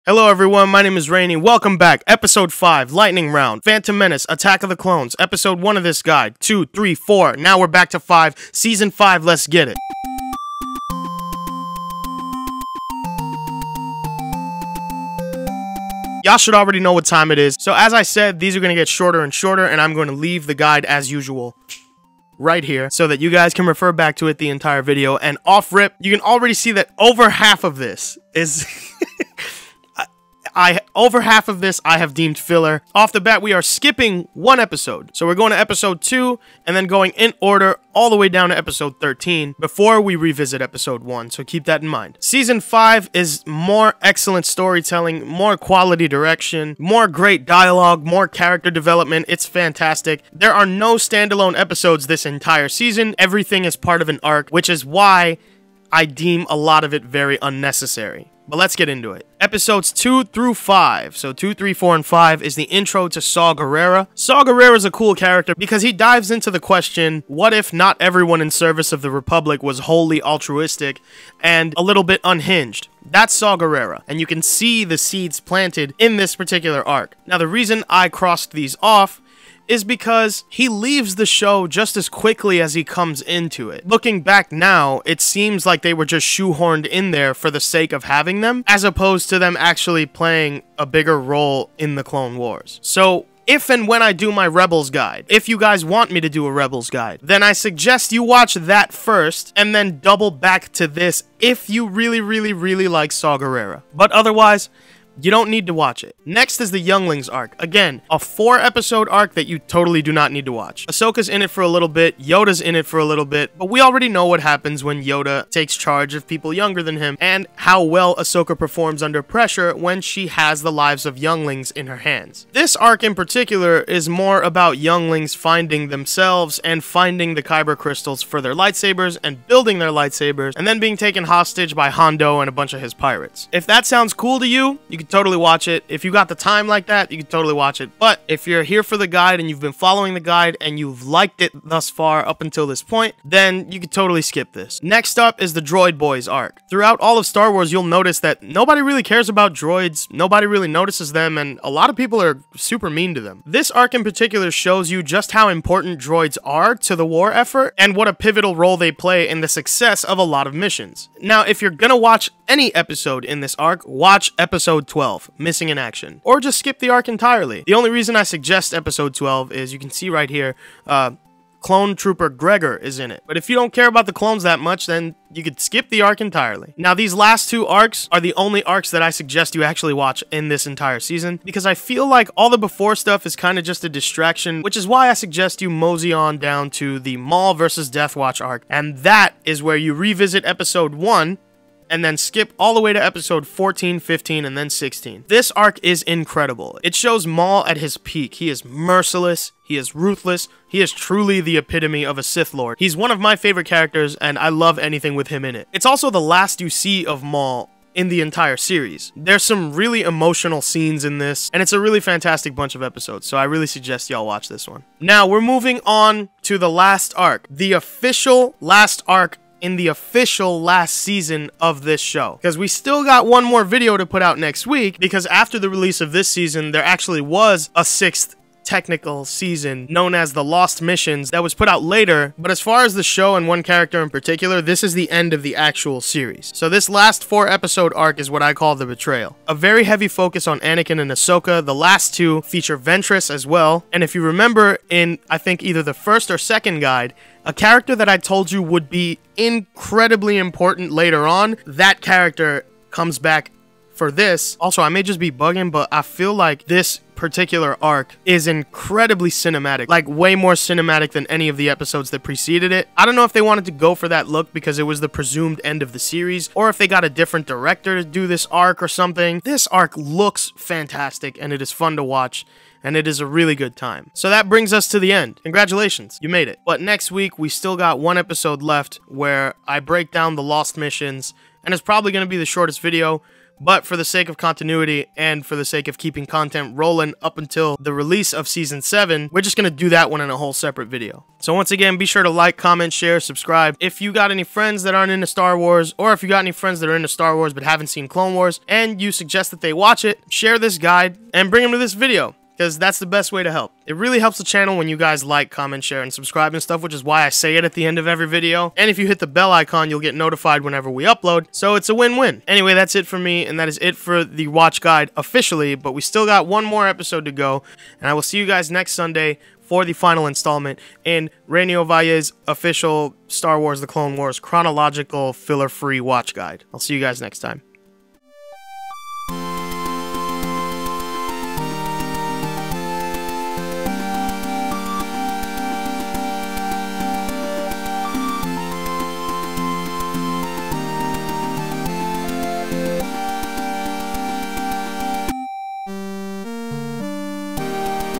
RAINEY's Guide to STAR WARS: THE CLONE WARS - Ep. V
HEARSE BOYS Presents: RAINEY OVALLE's official  FILLER_FREE Star Wars: The Clone Wars CHRONOLOGICAL watch-guide.

In this video series, as we approach season 7 of The Clone Wars on Disney+, RAINEY breaks down the correct order in which you should watch Cartoon Network's acclaimed series 'The Clone Wars.' Approaching the end, here with Part V, in this VI part series, every Sunday until the release of The Clone Wars. One more week!!!